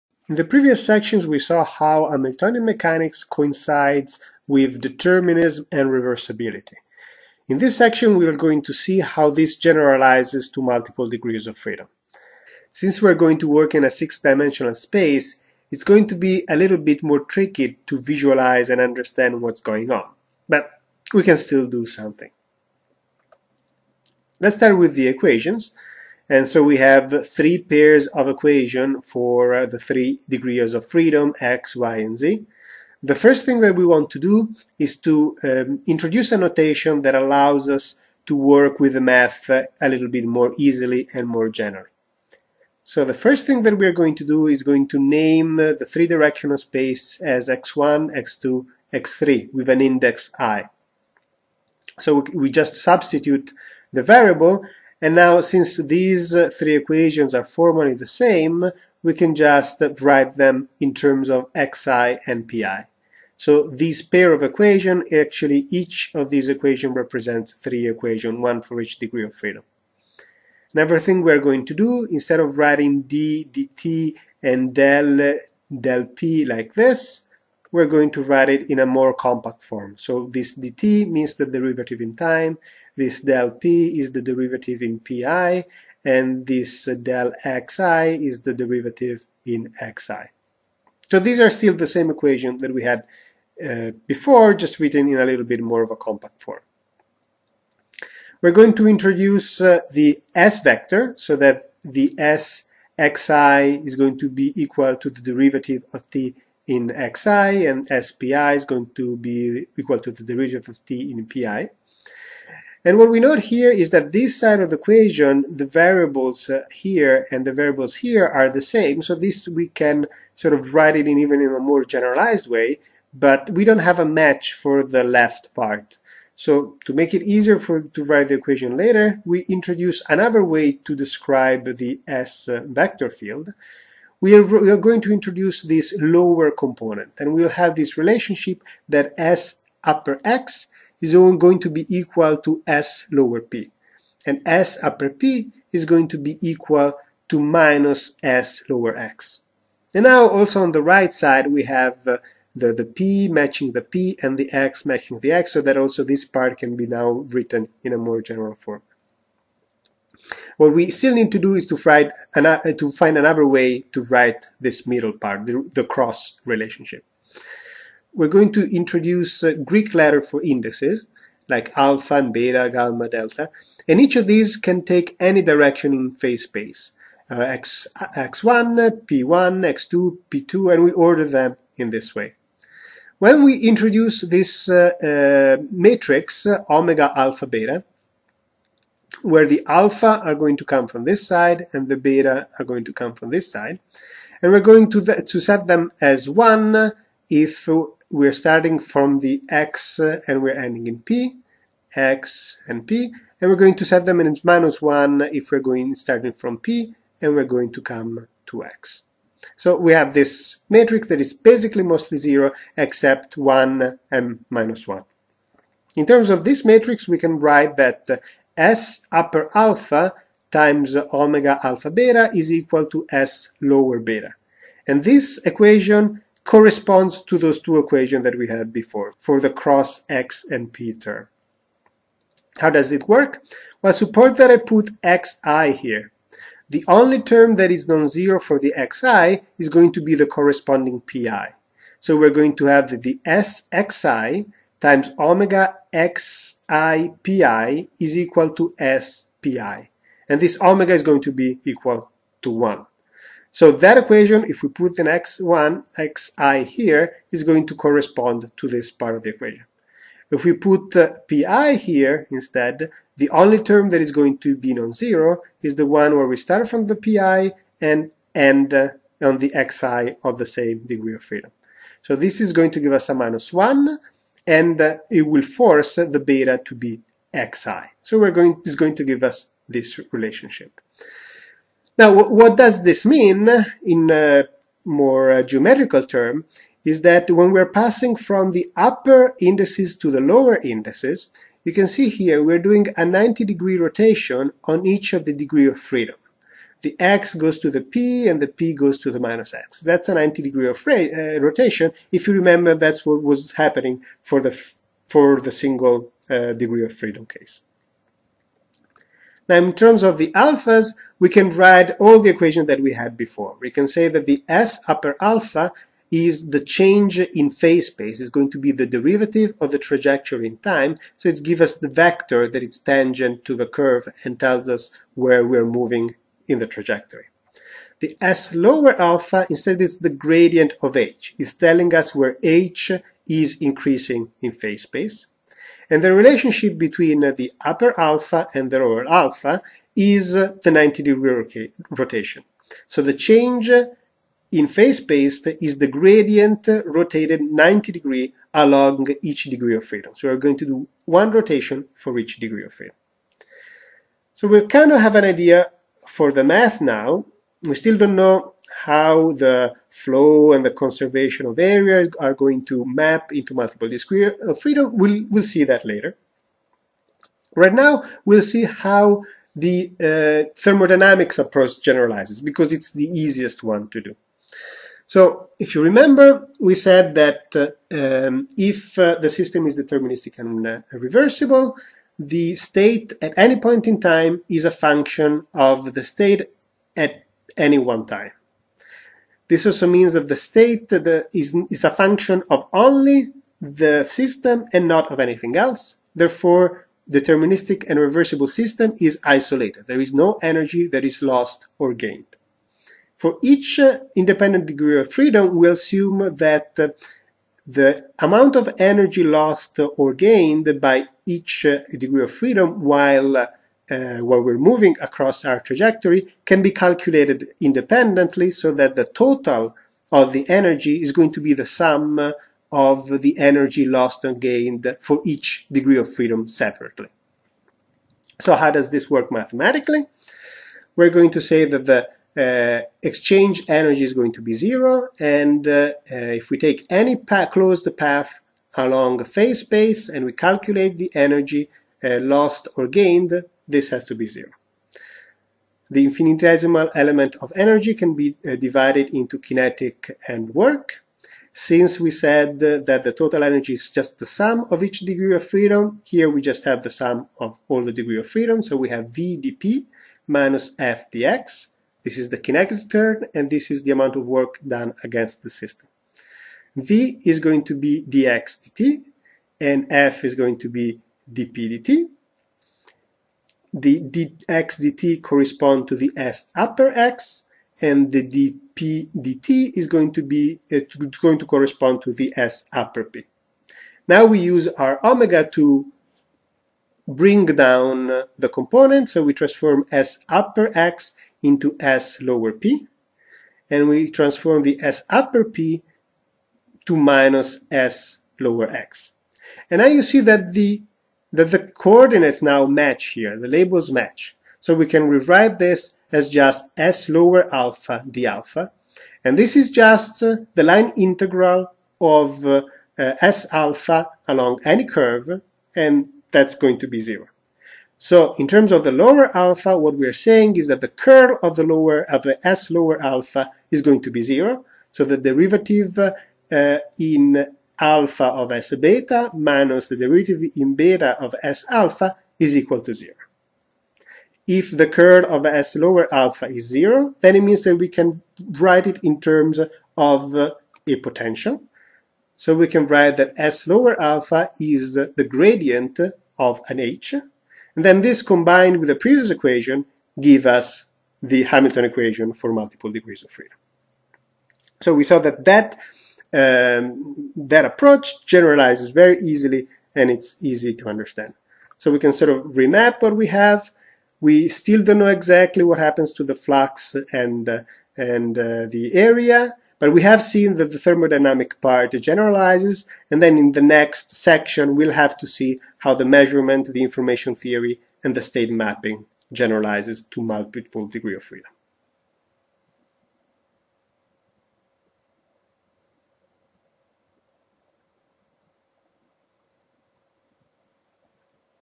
Understanding Hamiltonian mechanics: (6) Multiple d.o.f.
A different way to understand classical Hamiltonian mechanics in terms of determinism and reversibility. See all videos in the series: